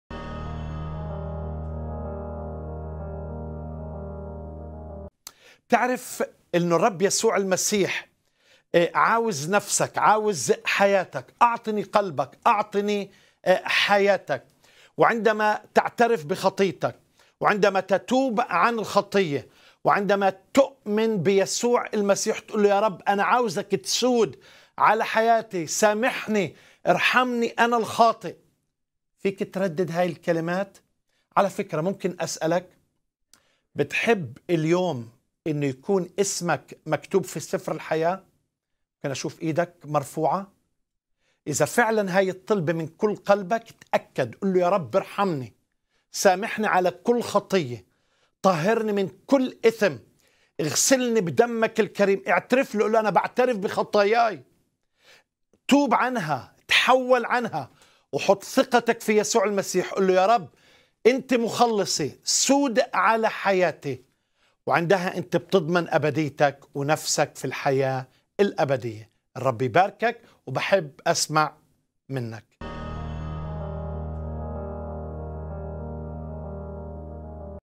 421 هل تحب أن يكتب اسمك في سفر الحياة؟
نفسك في الميزان

حلقة هذا الأسبوع بعنوان "نفسك في الميزان"

في هذه الحلقة يتحدث القس نزار شاهين عن قيمة النفس ويجيب على العديد من الأسئلة المصيرية الهامة

شاهد في هذه الحلقة :
ما هو مصدر النفس بحسب كلمة الله؟
ما هو السبب الرئيسي لتمييز النفس البشرية؟
هل يمكن أن يستوعب الإنسان الله بعقله المحدود؟
ما هي وظيفة النفس في شخصية الإنسان؟
ما هو غرض الله للإنسان وما هو دور المسيح في تحقيقه
هل هناك ارتباط بين خلق آدم وعقيدة الثالوث؟
لماذا شارك الله آدم في السيادة على المخلوقات؟
متى بدأ الله في التخطيط لفداء الإنسان؟
نفسك في الميزان - ما هي قيمة النفس؟
إنتبه .. عندما تولد نفسك ستحيا للأبد
نفسك غالية جداً على قلب الله
نفسك تساوي دم المسيح المسفوك لأجلك
ماذا تنتفع لو ربحت العالم كله وخسرت نفسك؟
انتبه .. أنت هدف ثمين يسعى إبليس لضربه
ما هو الدين الذي يستحل ويسفك دم الأبرياء؟
انتبه .. لا يكفيك أن تكون مسيحي بالاسم
صديقي .. الله يحبك والشيطان يريد تدميرك
هل تحب أن يكتب اسمك في سفر الحياة؟